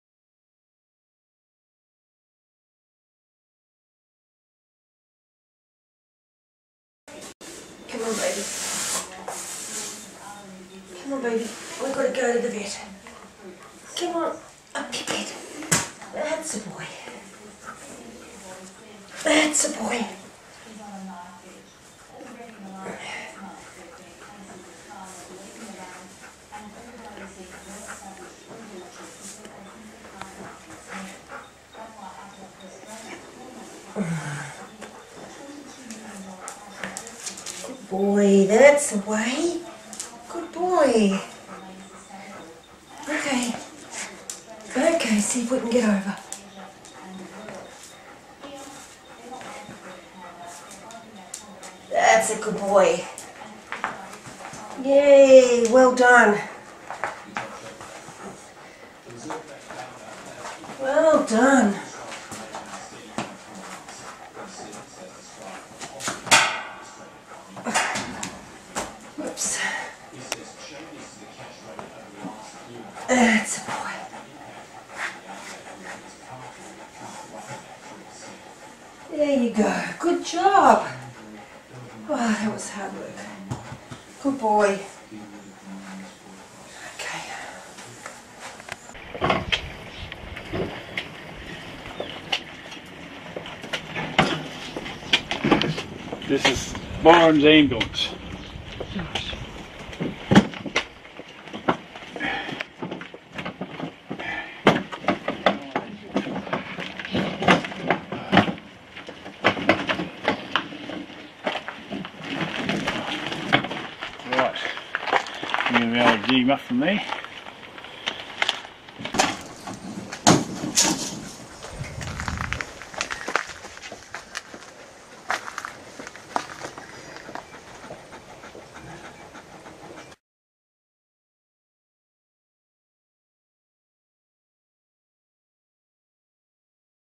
byron collapse
Byron had another collapse yesterday. This is how he is affected by the sudden onset collapse.  Running around one day, and unable to move the next, can't sit, can't stand, can't walk.  This is the 3rd Collapse of this nature that he has had, the last one was a few months ago.